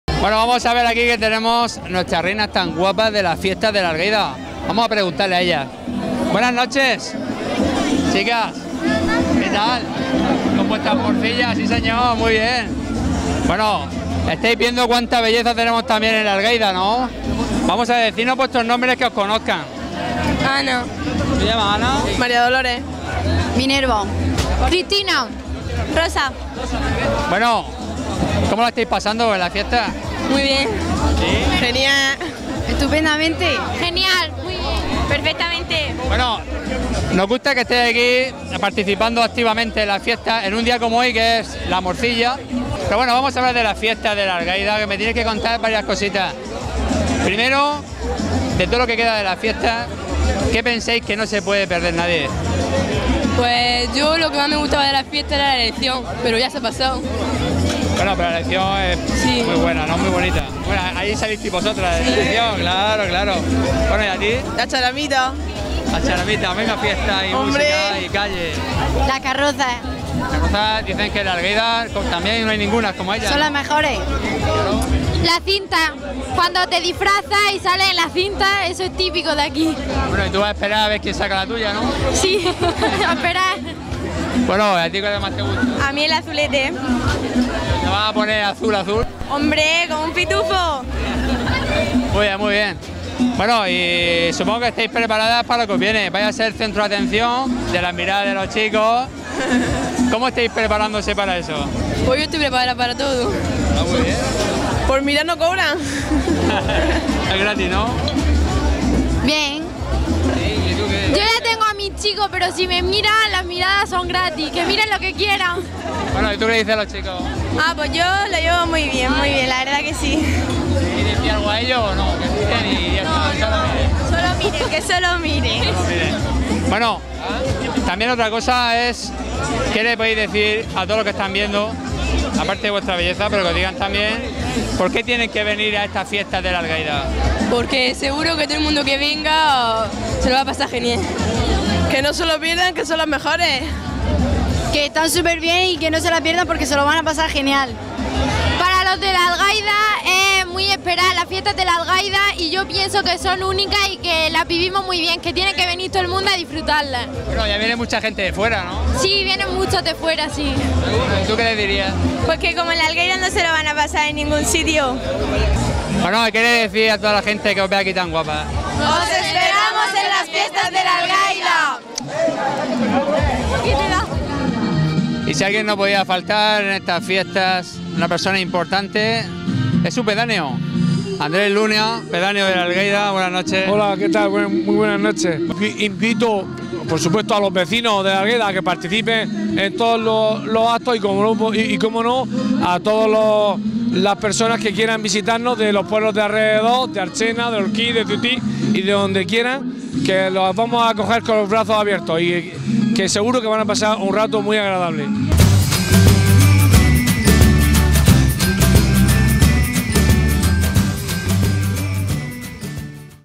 Video Reinas Fiestas La Algaida 2015. Chicas guapas que te invitan a la fiesta
Vídeo Reinas de las Fiestas La Algaida 2015. Chicas guapas que se presentan con su nombre, te mandan un beso, te dicen lo que más les gusta de las fiestas y además te invitan a sus fiestas que son hasta el 23 de Septiembre. Te explican por qué tienes que ir. También te invita el pedáneo de Archena, representando al ayuntamiento de Archena y la comisión de fiestas.

Última hora: "Accidente de tráfico grave hoy en Ceutí. Bomberos al rescate. Vecinos denuncian al ayuntamiento"